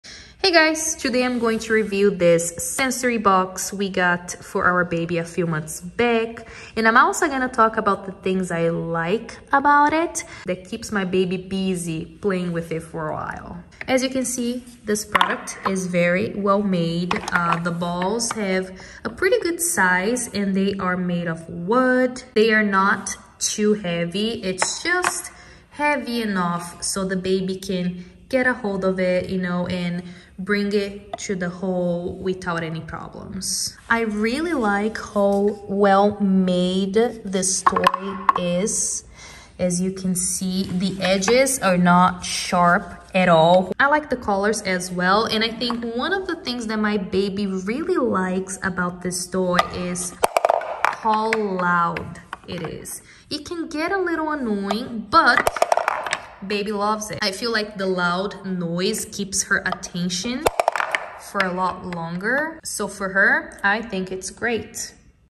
Adena Montessori Full Size Object Permanence Box Review - Is It Worth It?
Adena Montessori's Object Permanence Box is an ideal toy for 6-12 month infants, helping them understand object permanence and aiding cognitive growth. It’s more than a toy; it enhances hand-eye coordination and refines fine motor skills. Moreover, the use of differently colored balls promotes color recognition while the task of inserting balls in holes boosts focus and concentration. Crafted from safe, durable materials, it’s designed with your child’s wellbeing in mind. With this educational and fun toy, your infant’s development is in good hands.